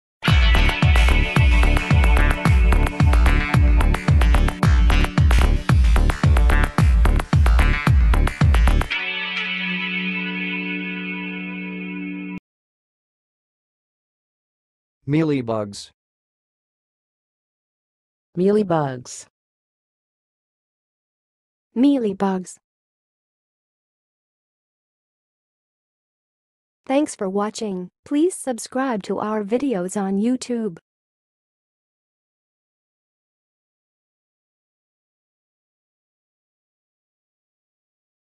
How to say "mealybug's"! (High Quality Voices)
Watch in this video how to say and pronounce "mealybug's"!